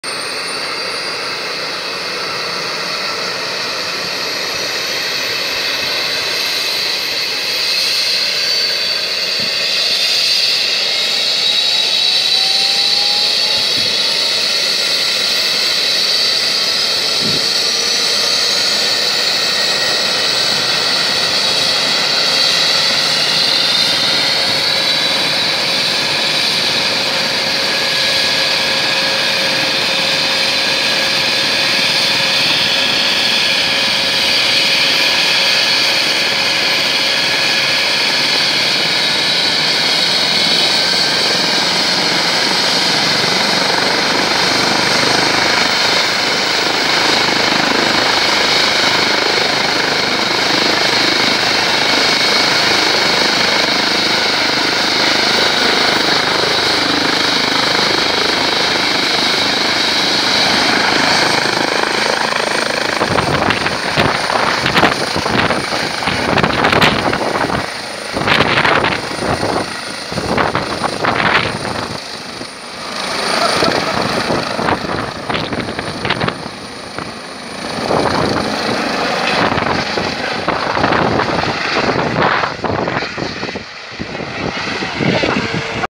Mountain rescue helicopter
Saw this in alps while skiing